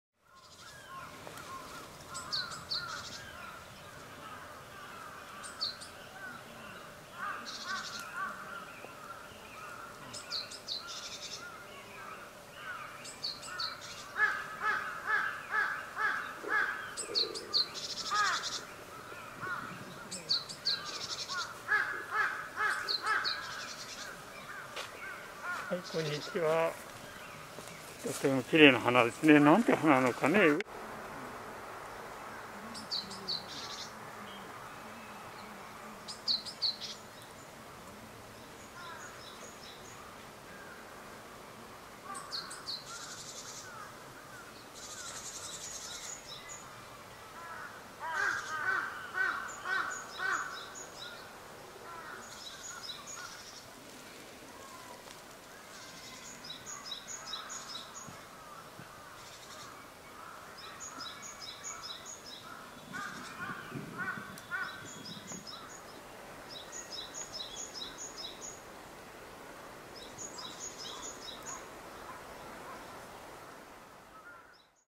アマドコロ/Polygonatum odoratum 02_210425_ガイコツ山
アマドコロか別物かわからないままデータを追加していく適当さが当方の形。茎が横を向くのと、割とまっすぐ上に伸びるものが数メートルの距離と1週間ほどの発芽時期の遅速を隔てた群れが横並びにあり、これは後者、割とまっすぐ立っている群れ。散歩中の人がフレームに入ってしまったが、コロナの時節柄マスクをされていたので肖像権は問題ないだろう。
yasou.jp,plant,ユリ科,アマドコロ属,アマドコロ,Polygonatum odoratum,ガイコツ山,